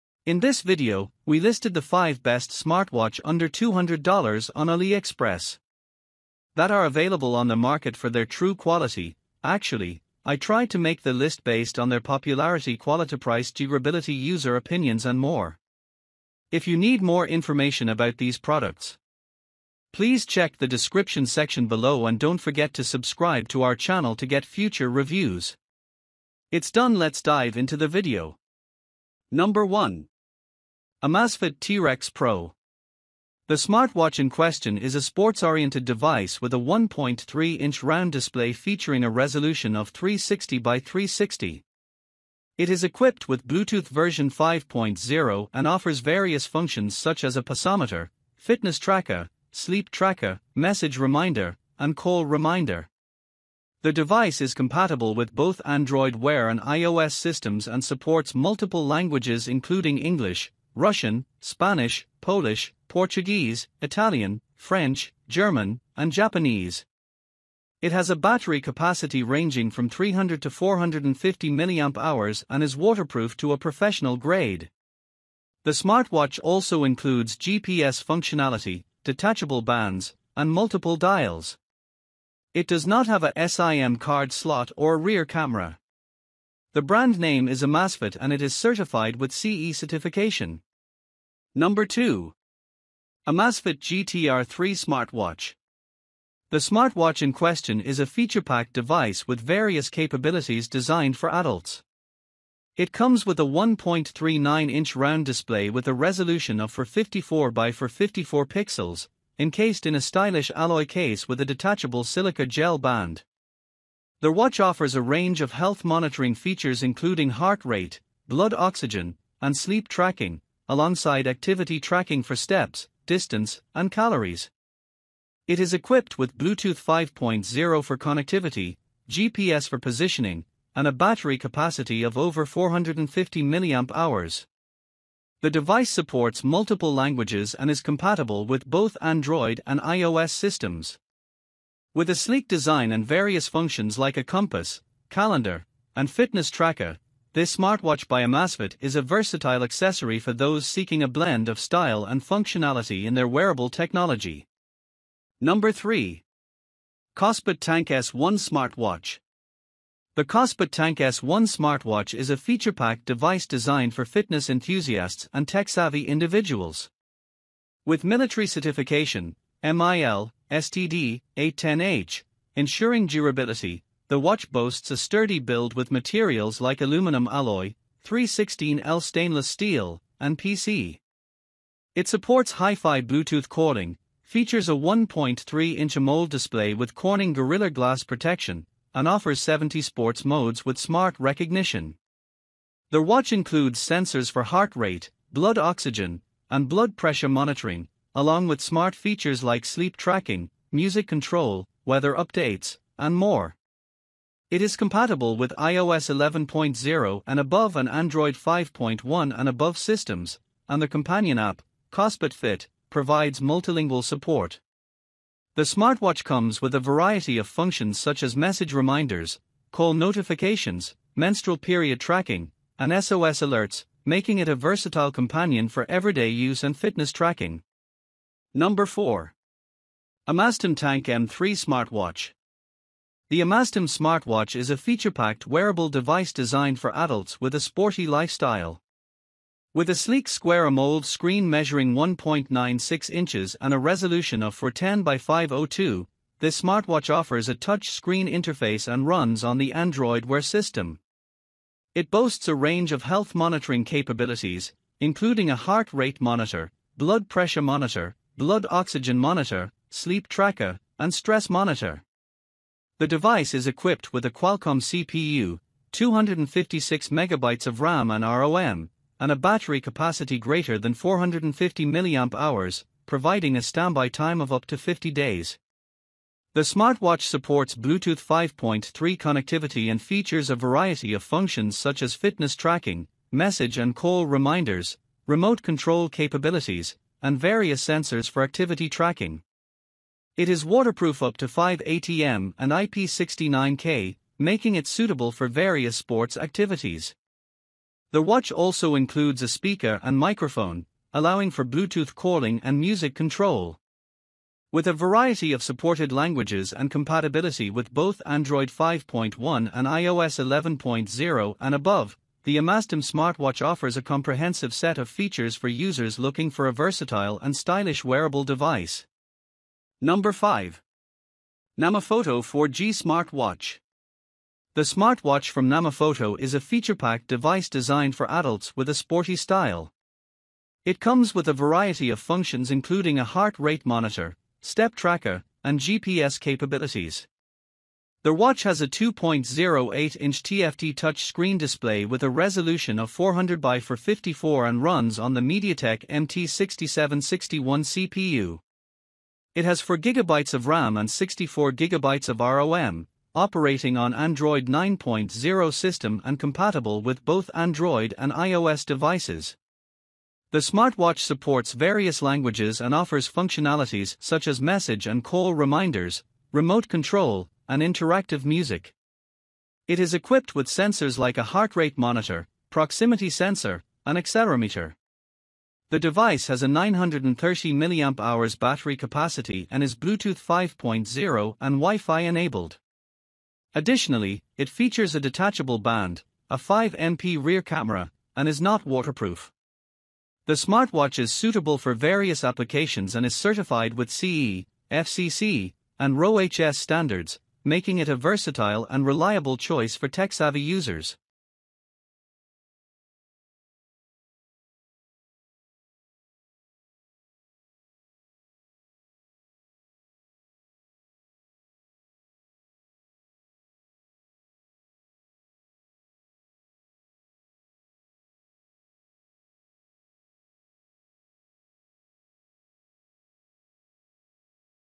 ✅Best smartwatch Under $200 On Aliexpress | Top 5 smartwatch Reviews
Amazfit T-Rex Pro

Amazfit GTR 3 Smartwatch

KOSPET TANK S1 Smartwatch

AMAZTIM TANK M3 Smartwatch

NAMOFOTO 4G Smart Watch

Hey guys in this video we are going to be checking out the Best smartwatch Under $200. You Can Buy Right Now. We made This List Based On Our Personal Opinion and hours of research & we have listed them based on the type of features and Price. We have Included options for every type of user so whether you are looking for the best budget.

best cheap smartwatch for android

      owners.